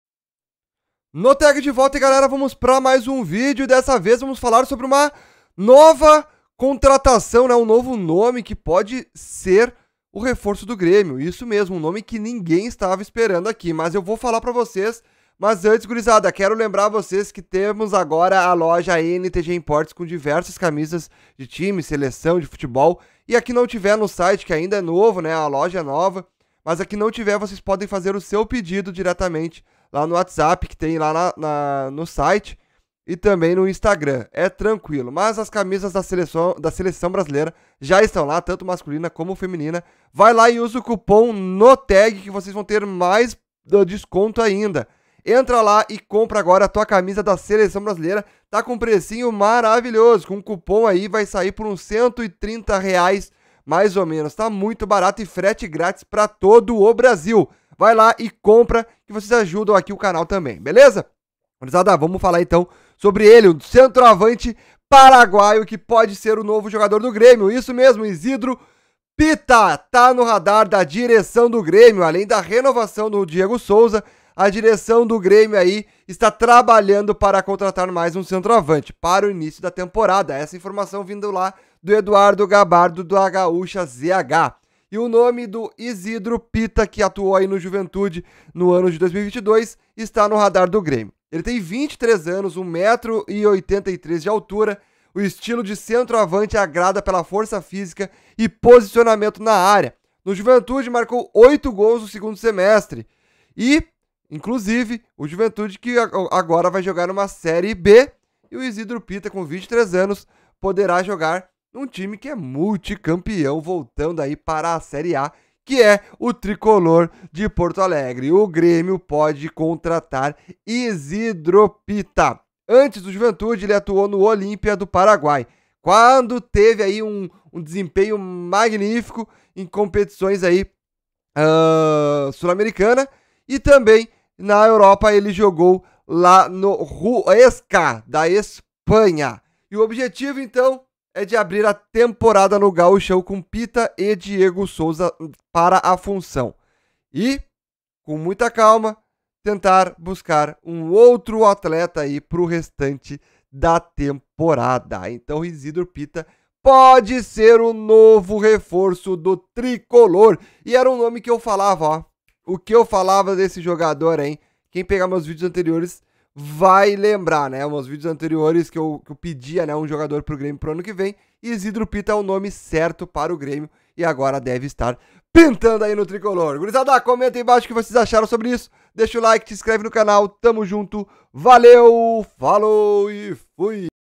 URGENTE! NOVO REFORÇO PINTANDO NO GRÊMIO, CENTROAVANTE PARAGUAIO PODE COMPOR O TIME DO TRICOLOR!
(Use o cupom NOTAG)